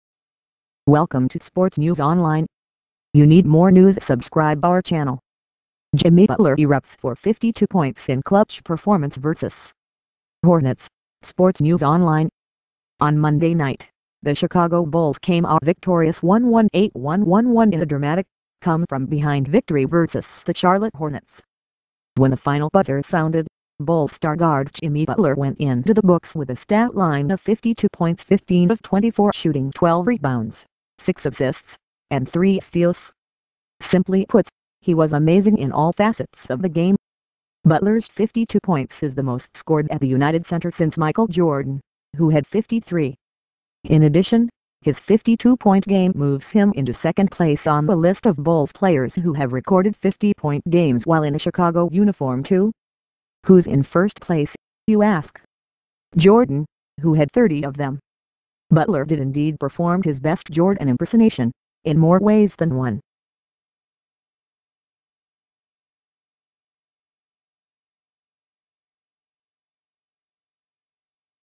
Jimmy Butler erupts for 52 points in clutch performance vs Hornets , Sports News Online
On Monday night, the Chicago Bulls came out victorious 118-111 in a dramatic, come-from-behind victory vs. the Charlotte Hornets.

When the final buzzer sounded, Bulls star guard Jimmy Butler went into the books with a stat line of 52 points (15 of 24 shooting), 12 rebounds, 6 assists, and 3 steals. Simply put: He was amazing in all facets of the game:

Butler’s 52 points is the most scored at The United Center since Michael Jordan, who had 53.

In addition, his 52-point game moves him into second place on the list of Bulls players who have recorded 50-point games while in a Chicago uniform (2). Who’s in first place, you ask? Jordan, who had 30 of them.

Butler did indeed perform his best Jordan impersonation, in more ways than one…